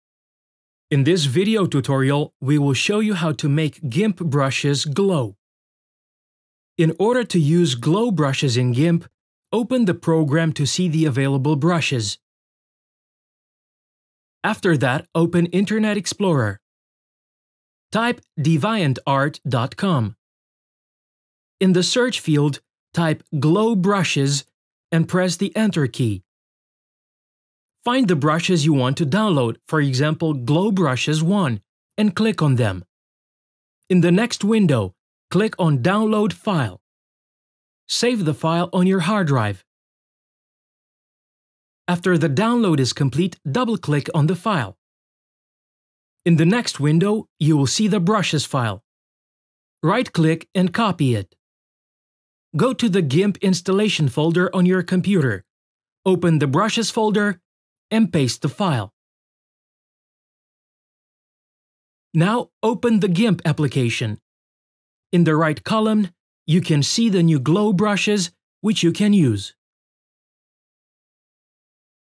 How to Make GIMP Brushes Glow
Learn how to download and install glow brushes in GIMP editor.

In this video tutorial we will show you how to make gimp brushes glow.
In order to use glow brushes in GIMP, open the program to see available brushes. After that, open Internet Explorer. Type "deviantart.com". In the search field, type "Glow brushes" and press "Enter" key. Find brushes you want to download, for example "Glow Brushes I", click on them. In the next window click on the "Download file".

Save the file on your hard drive. After the download is complete, double-click on the file. In the next window you will see the brushes file, right-click and "copy" it.

Go to the GIMP installation folder on your computer, open the "brushes" folder and paste the file.
Now, open the GIMP application. In the right column you can see new glow brushes, which you can use.